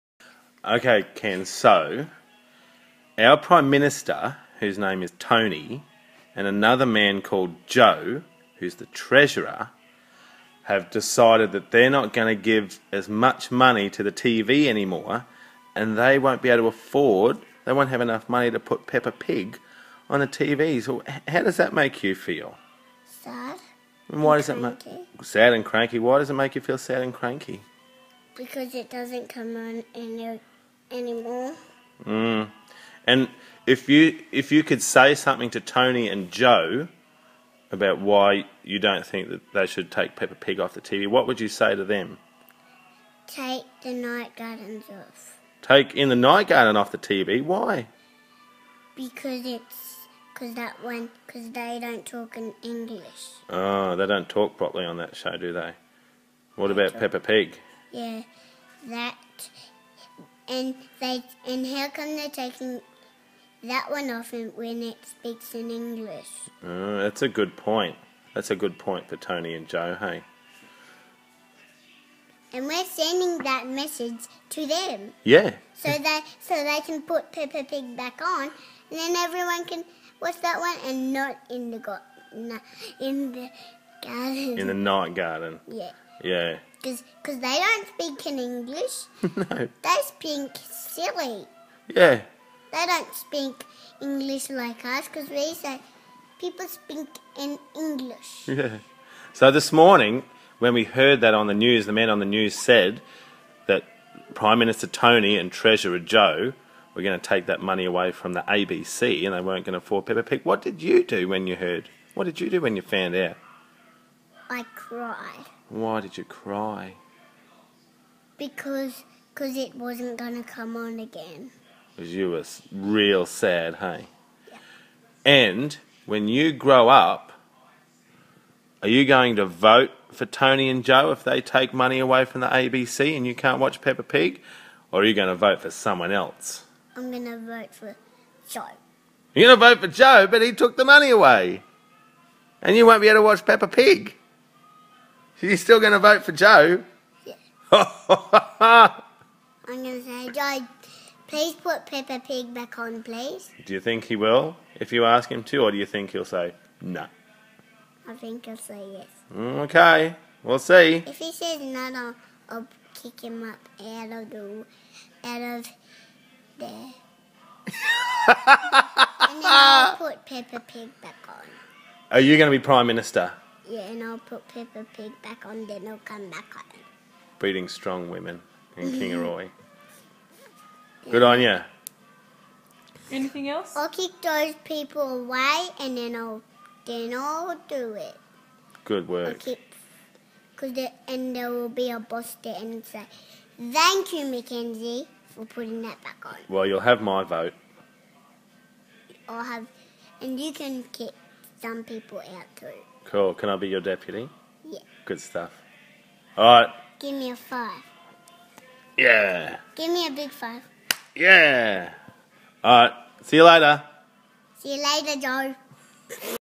A message to the Treasurer Joe Hockey
The Liberal Abbott Gov Budget is going to hit all Australians hard, the latest victim being my 4 yr old daughter who cried this morning when she heard on the news that Peppa Pig may be axed due to proposed funding cuts to the ABC.